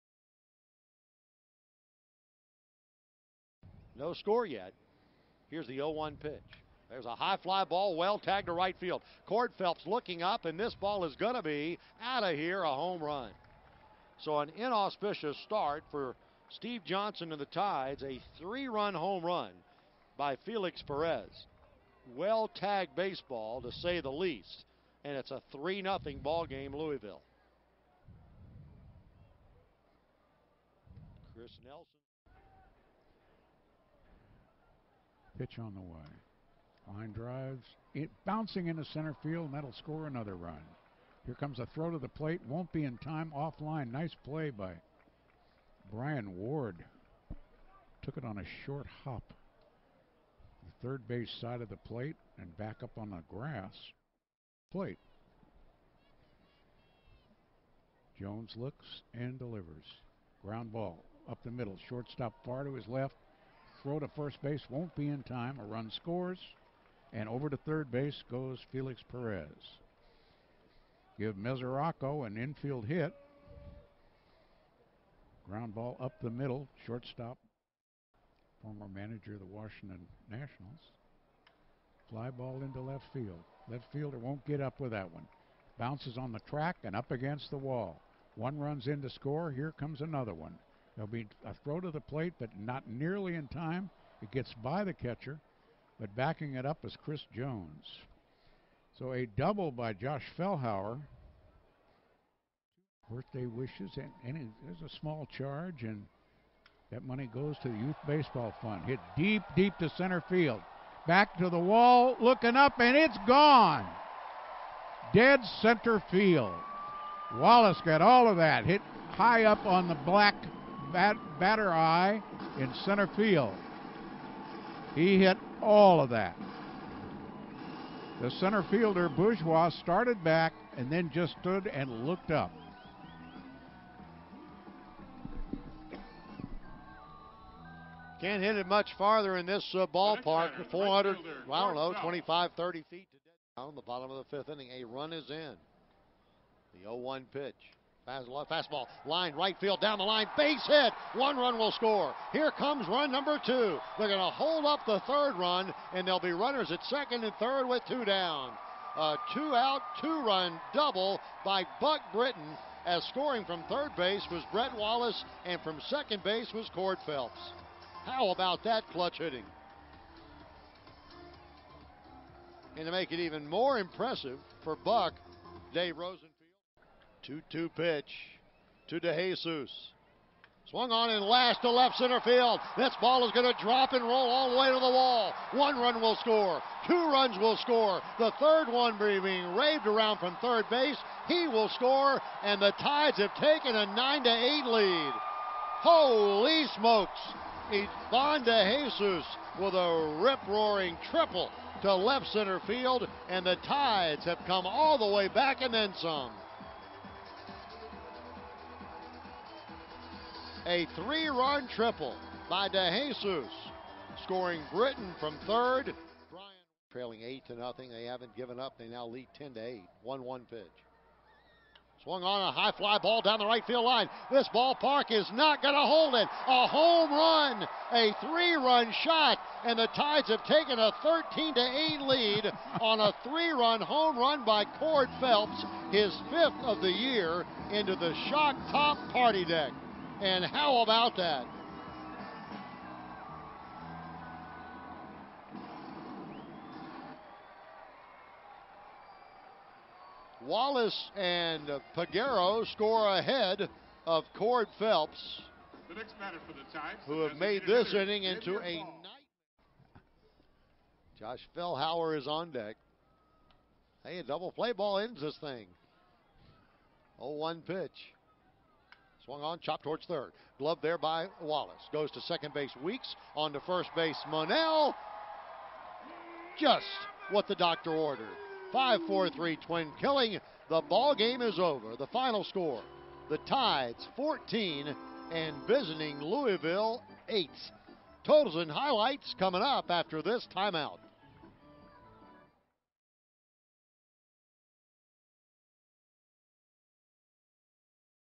May 13th - Norfolk 14, Louisville 8
Despite trailing 8 to 0 heading into the bottom of the third inning, the Norfolk Tides scored 14 unanswered runs as they defeated the Louisville Bats 14-8 Tuesday night at Harbor Park.